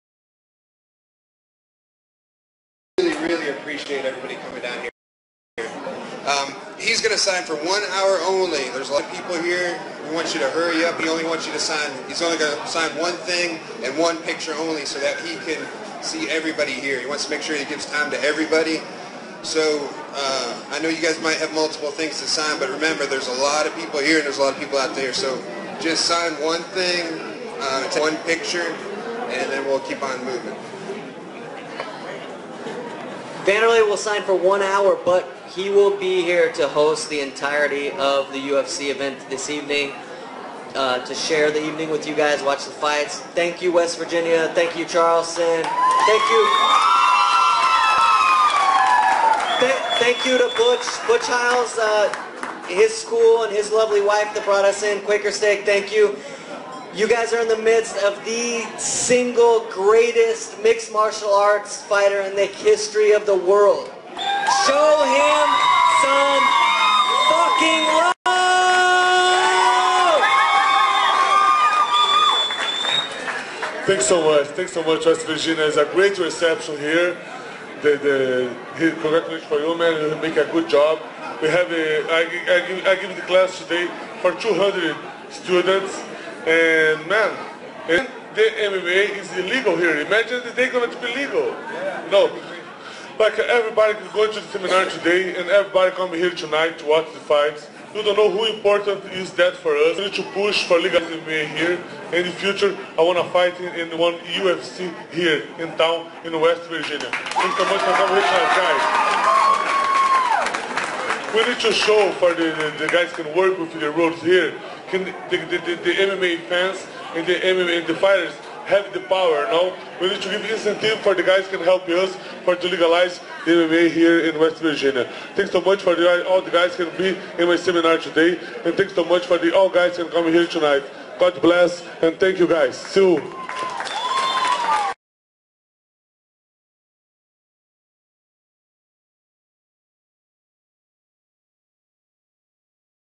Wanderlei Silva Says WV Needs to Legalize MMA and He Wants to Fight in WV
Wanderlei Silva and Butch Hiles go to Quaker Steak & Lube to announce that they want to leagalize MMA in WV and bring a UFC here with him in it!!!